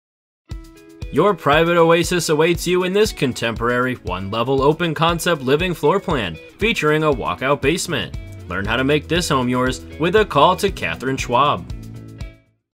21 Lang Drive North Kingstown, RI | ColdwellBankerHomes.com
21 Lang Drive, North Kingstown, RI

Situated within the desirable Wickford Highlands neighborhood area, your private oasis awaits you in your Contemporary Ranch’s one-level living floorplan. As you enter through the front door, be greeted by the lovely foyer and beautiful wide-planked hardwoods that lead you to the open-concept living